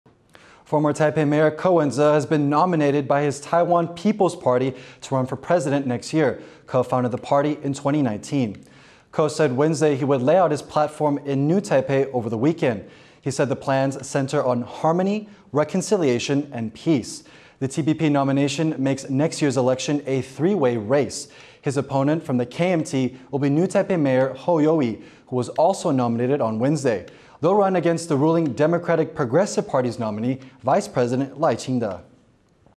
Former Taipei Mayor Ko Wen-je To Run for Party He Founded | TaiwanPlus News
📹Reporter(s): John Su/Sam Hui

Ko Wen-je, the former mayor of Taipei, will run for president of Taiwan on behalf of the Taiwan People's Party, the party he founded in 2019.